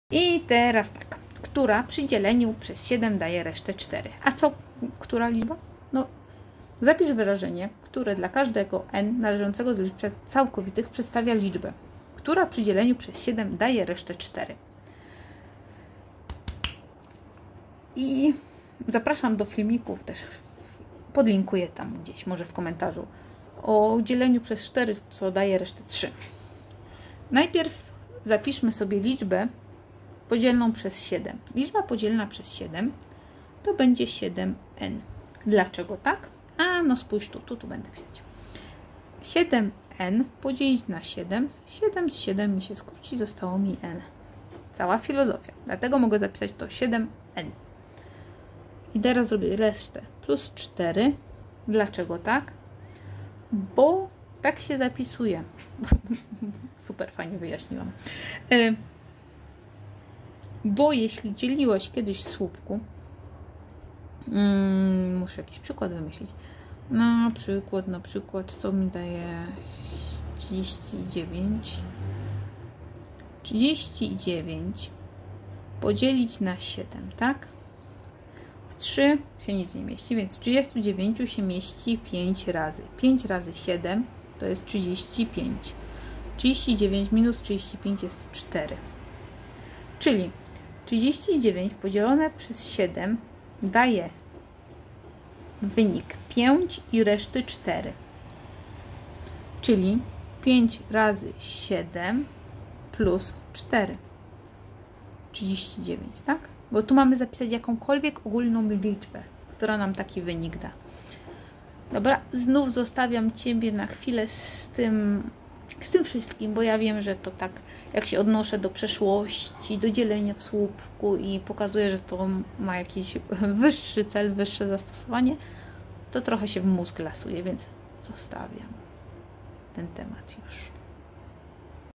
Zapisz wyrażenie, które dla każdego n należące do C przestawia liczbę, która przy dzieleniu przez 7
Zapisz wyrażenie, które dla każdego n należące do C przestawia liczbę, która przy dzieleniu przez 7 daje resztę 4.

I co teraz? :)

Tłumacząc zadania wychodzę z założenia, że potrafisz dodawać, odejmować, mnożyć i dzielić, Znasz ułamki i wiesz jak na nich powyższe działania działają, oraz potrafisz rozwiązywać proste równania.

Zadania rozwiązuję na żywca, tzn. nie nagrywam po kilka razy.

Tylko raz.

Dlatego czasem się zająknę, coś podumam :) i błąd też się znajdzie ;)